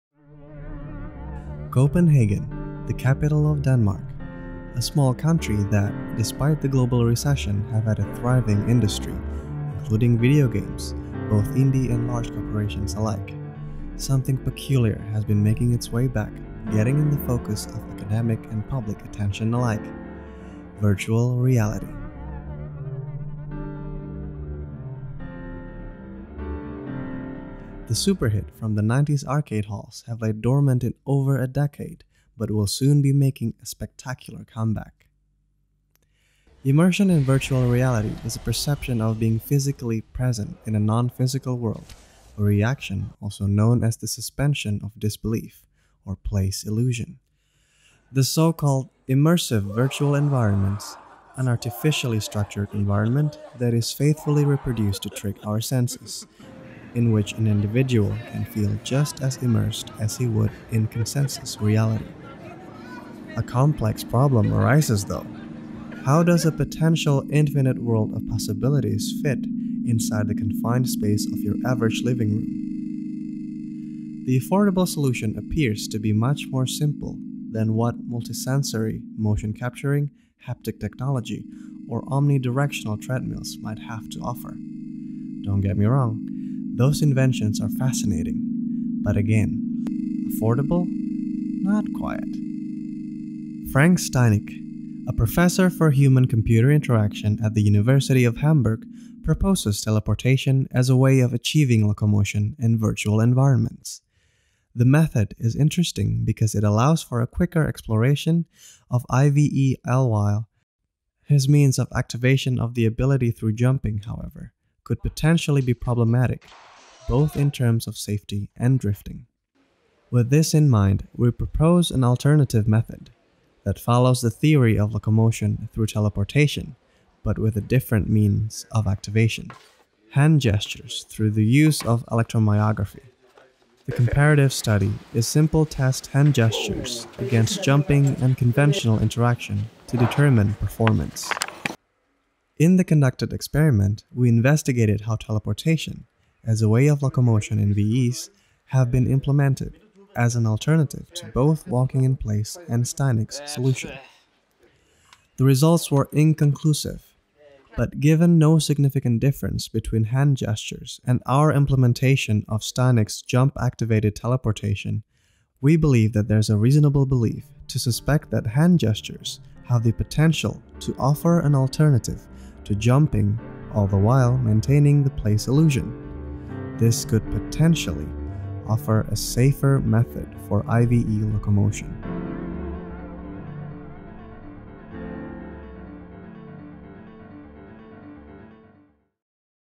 EMG-Controlled Teleportation For Immersive Virtual Environments
Constraint in a virtual environment is a topic that is often addressed when discussing Virtual Reality, in this paper we propose a method for navigating through a virtual environment through the use of hand gestures. After conducting research a questionnaire was devised to measure the participants feeling of presence in relation to Hand-gesture locomotion, compared with different locomotion techniques. The test was set up with three conditions: Hand Gesture, Jumping, and Clicking,  with a questionnaire following each condition. The results indicate that when compared to clicking, Hand Gesture has the potential to provide a method of locomotion with increased presence. When compared to jumping, there was no significant difference between the methods of locomotion.